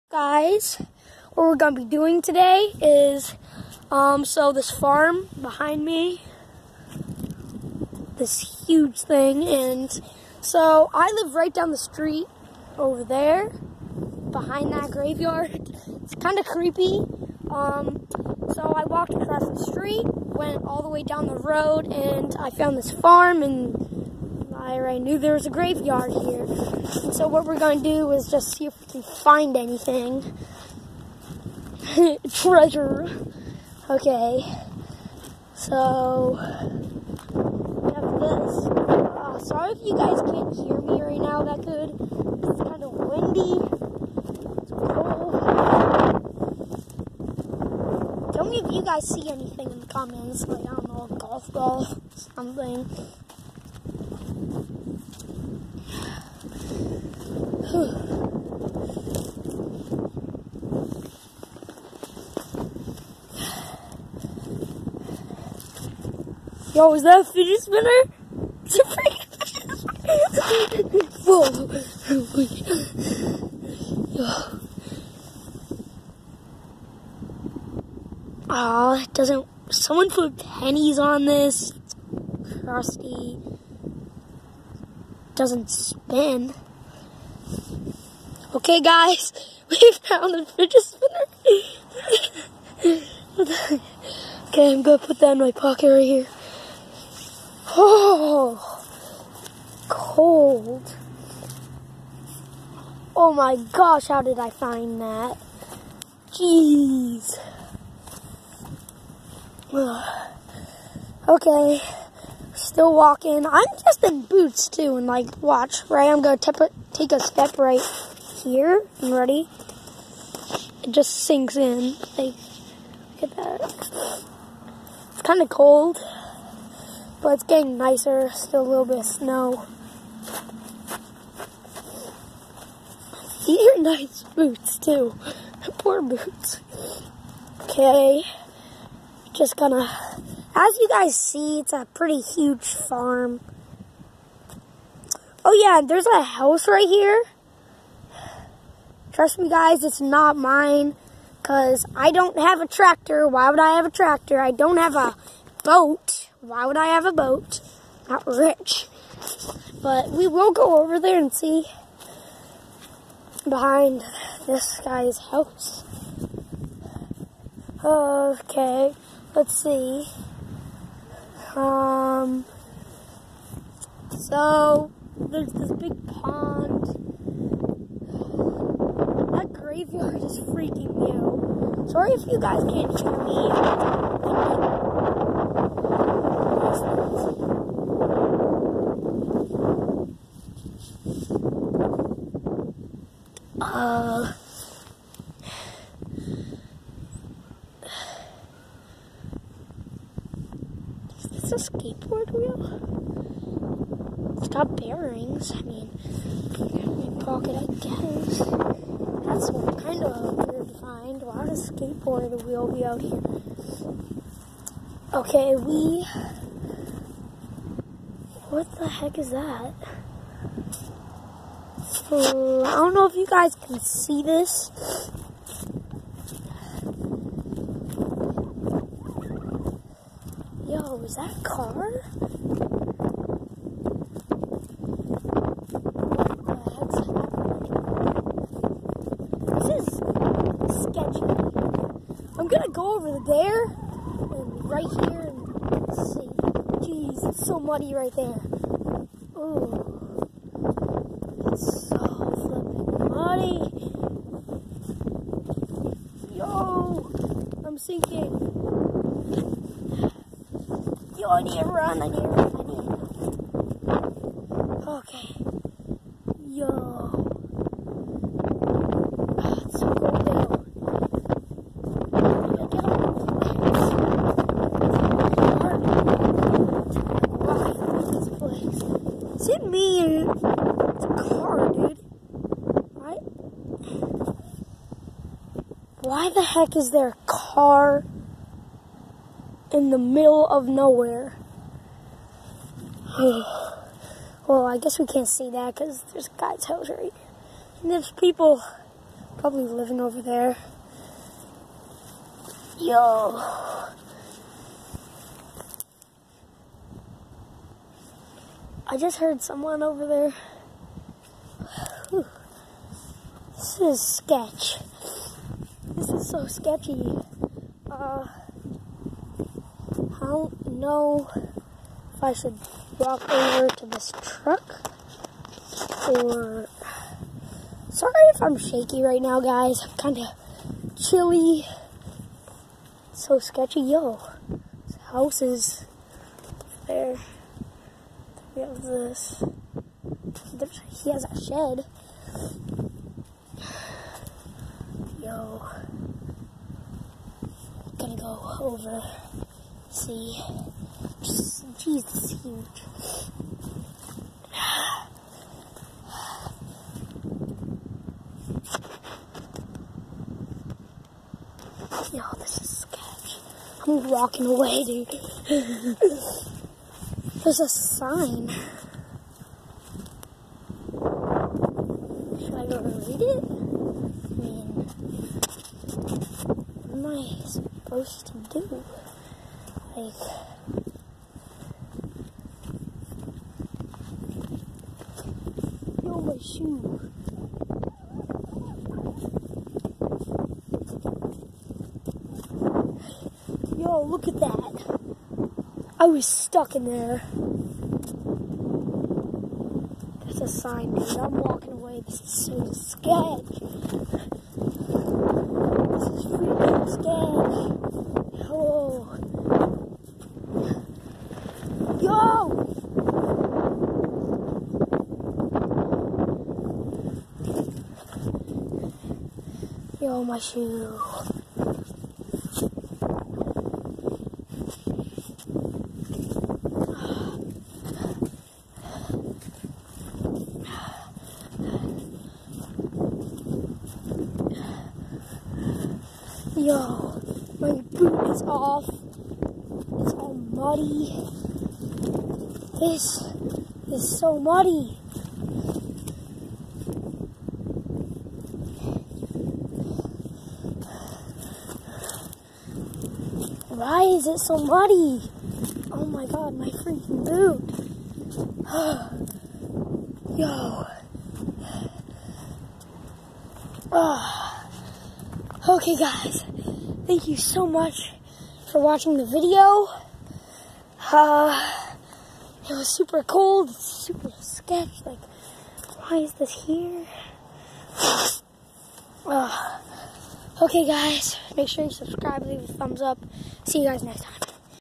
Found fidget spinner in haunted farm
It is me discovering a haunted farm